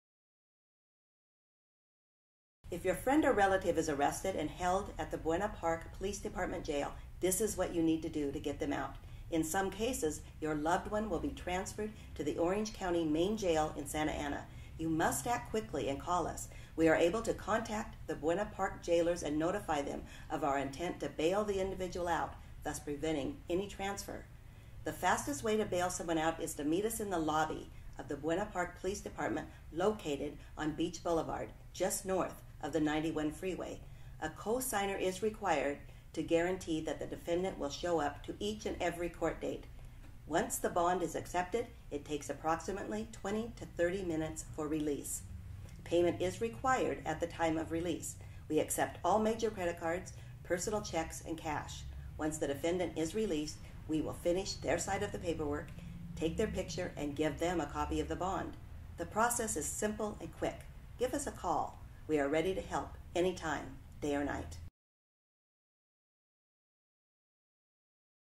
Watch Our Video Now To Learn About The Best Bail Bonds in Buena Park
Watch this video if you are looking for bail bonds in Buena Park. We are professionals in the bail bonds business, and we’re ready to serve you quickly and efficiently.

It is important that you know the facts and are assisted in getting things in order, should you be in the uncomfortable situation of having to bail out a loved one.

The first thing to know is that time is critical. Everyone is initially taken to the Buena Park Police Department, but they will be transferred to the county jail in a matter of hours. We need to call on your loved one’s behalf to let them know our intent to bail them out to prevent their transfer, which would delay their release by hours if not overnight.

You will have to fill out paperwork, provide a co-signer, and make a payment. We have everything covered to help you dot the “I’s” and cross the “T’s”, and we make your payment easy by accepting all major credit cards, cash, and even personal checks.

We know it is a worrisome situation, and we understand your discomfort, but we also know that things can get a lot better right away.